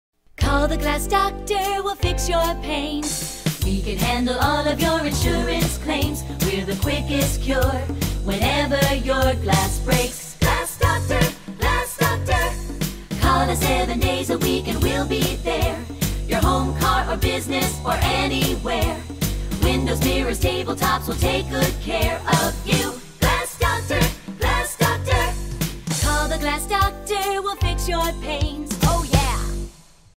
Home Windows, Auto Glass, Business Storefronts, We Fix Your Panes!
Whether your home needs window repair or replacement, your car needs a new windshield, or your business needs a new storefront, at Glass Doctor We Fix Your Panes!